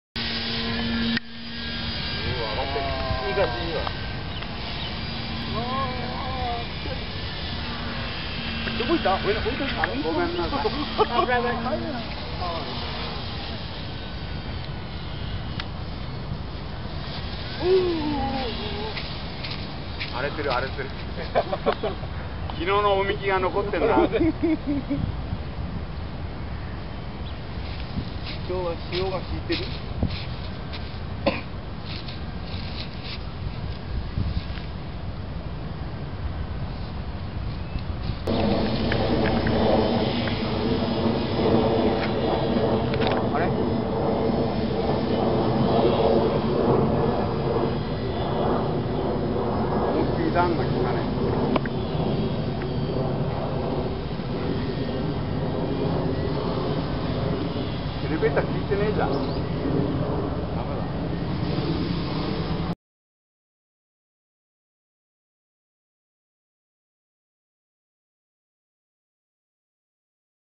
RC~大暴れASW28
ナゼカエレベーターが利かない。３回目の飛行で、コネクターが発進後に外れてしまったのです。アップの状態のまま～スグにスロットルを切り、無事に着陸しましたが、もうダメかと思いました。　　　　菅野隊撮影